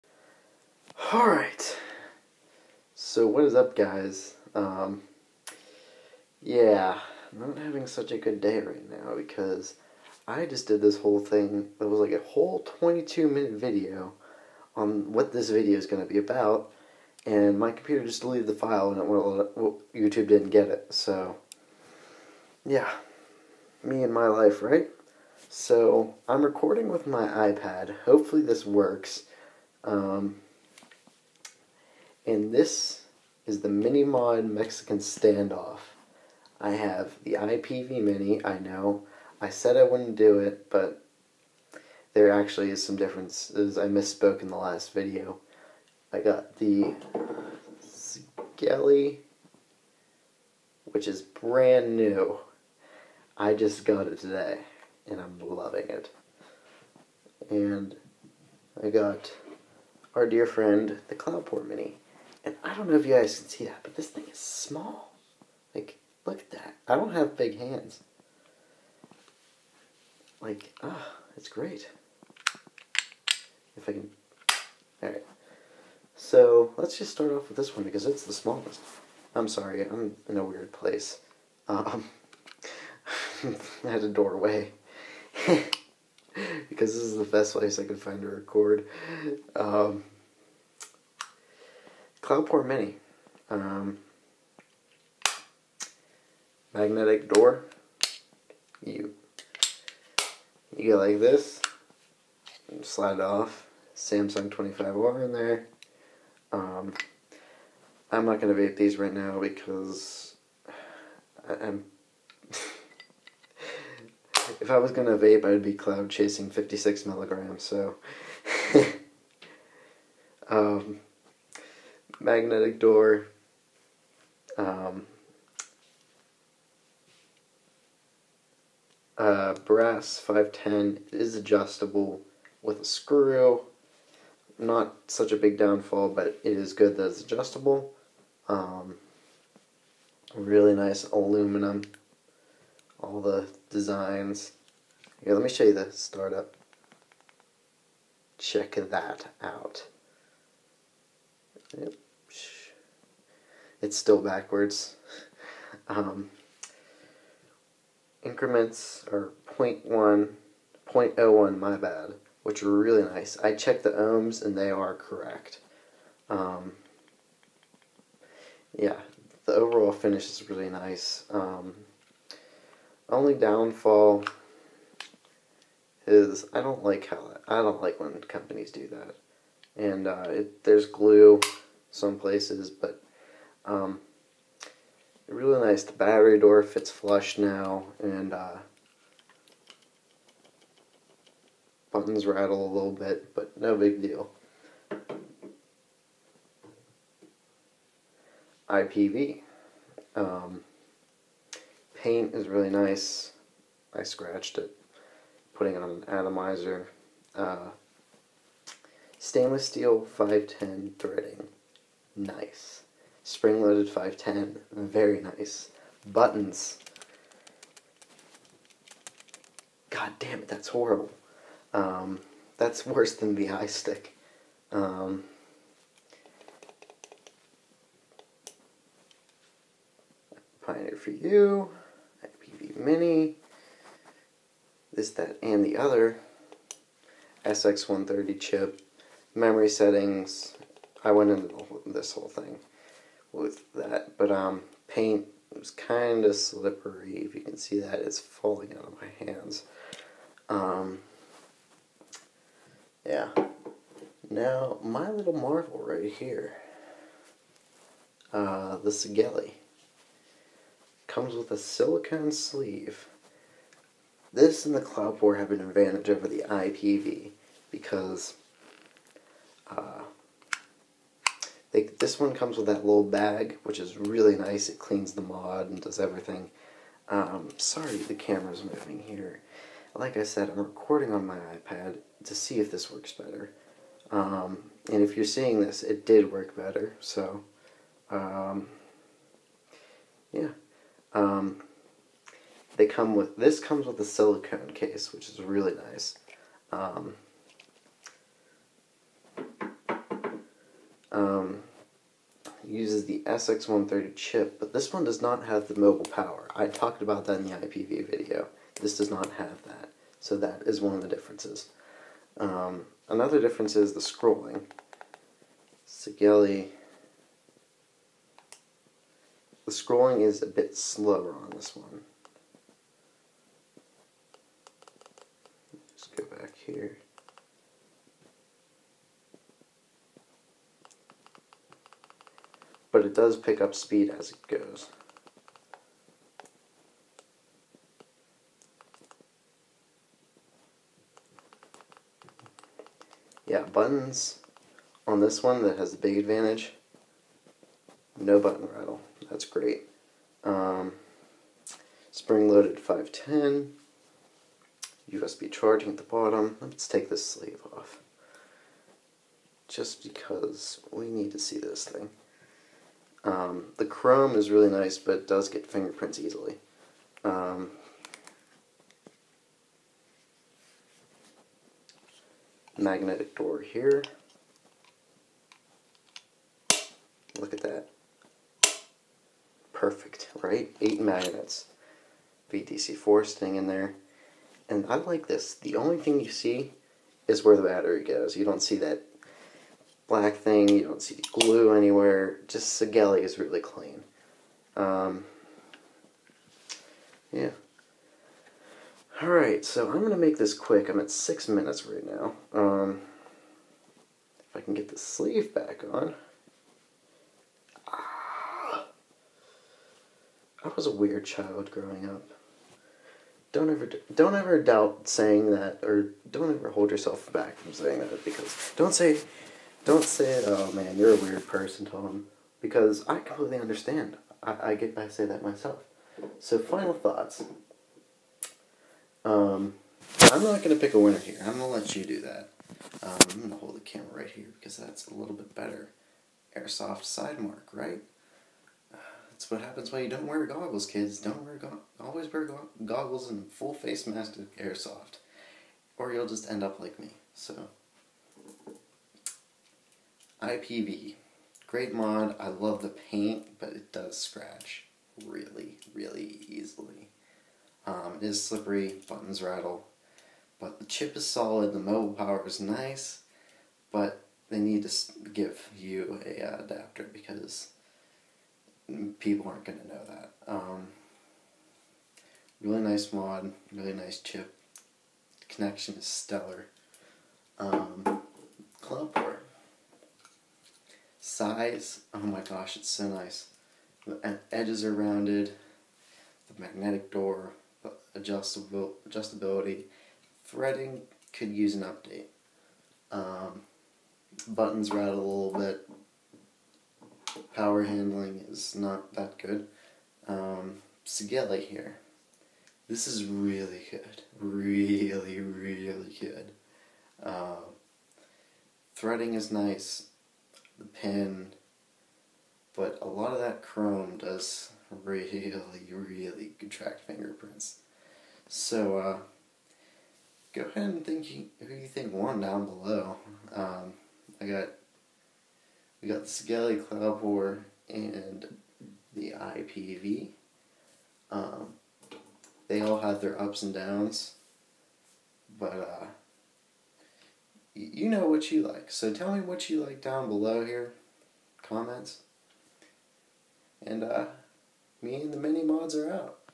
MINI MOD MEXICAN STANDOFF (IPV mini vs Sigelei mini vs Cloupor mini)
Oh complications, Here we take a quick comparison look at the popular 30 watt mini mods. I will go in depth more with the cloupor and sigelei once my computer is running better.
IPV: (ebay)